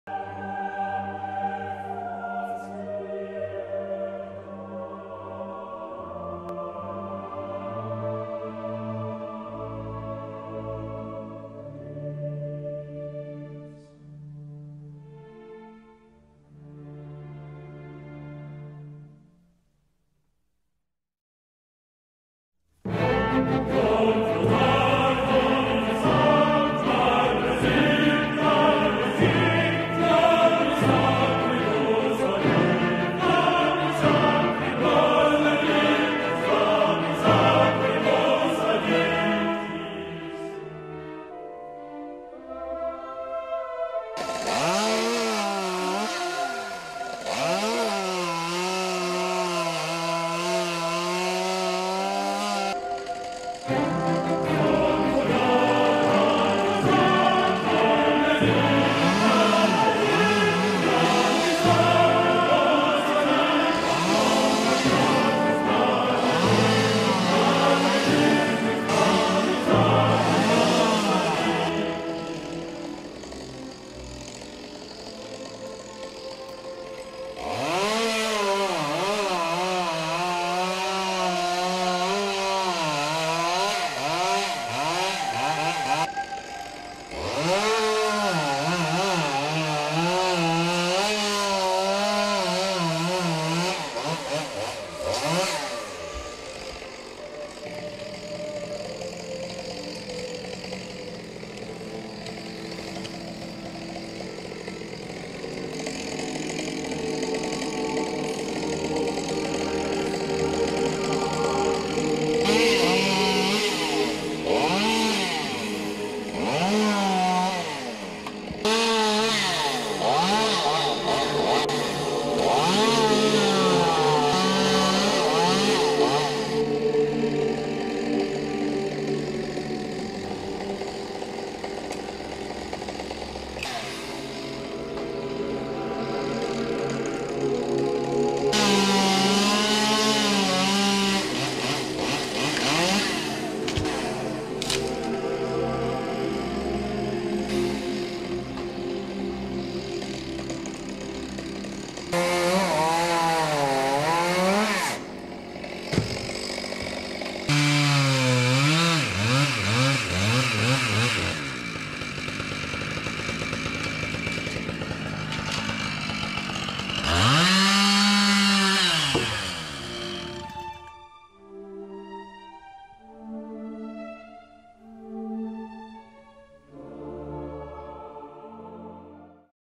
dood van een boom
de bomen in onze tuin worden gerooid